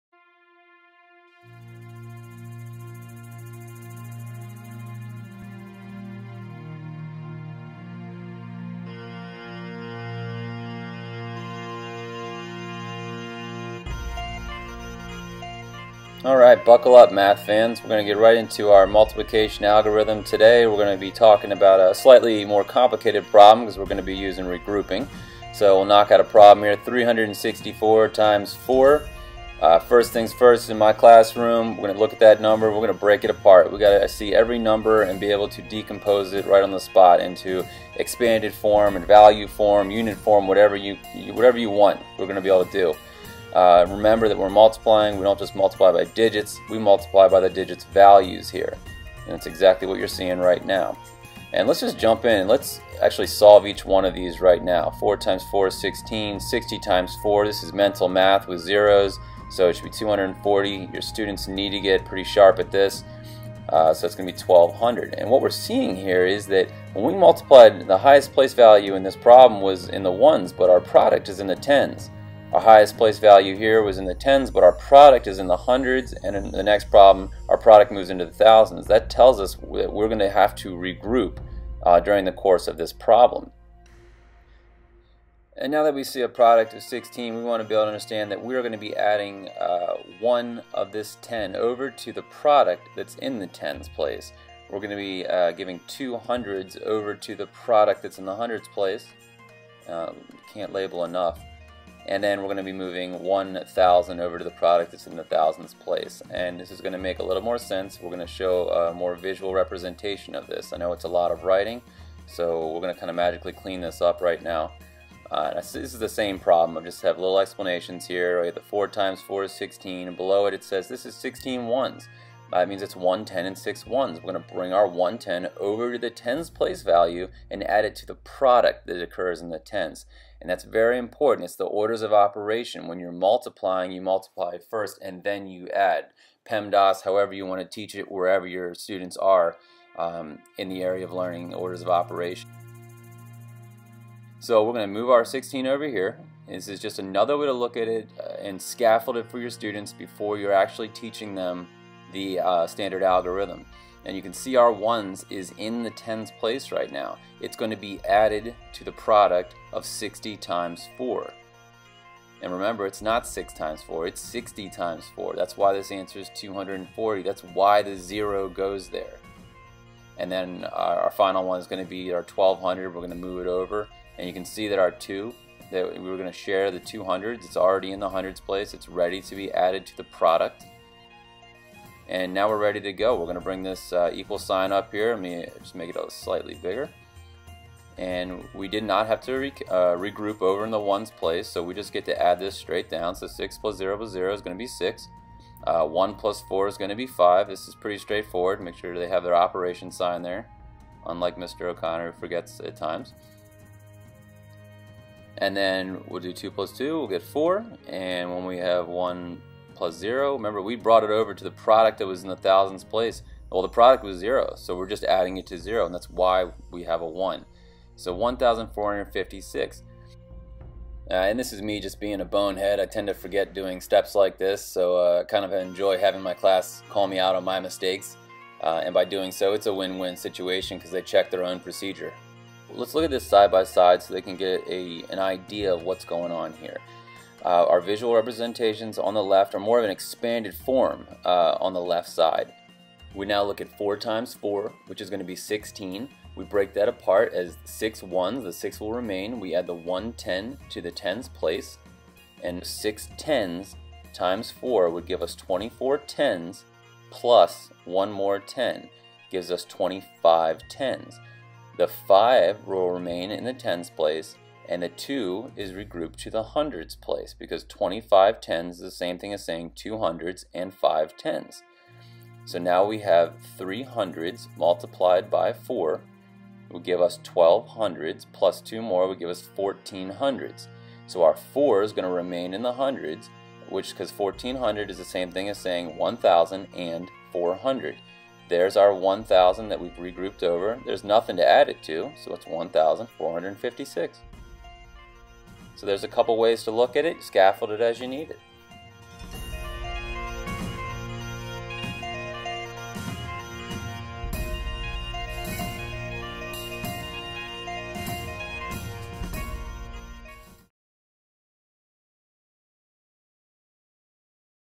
Multiplication: Explaining the Algorithm with Regrouping.
Video #2 takes another look into explaining the multiplication algorithm, but this time we introduce regrouping.  This video can be watched by students in the classroom as well as at home to assist them with homework.  Give it a try, use it to reinforce all the hard work you have given inside the classroom walls.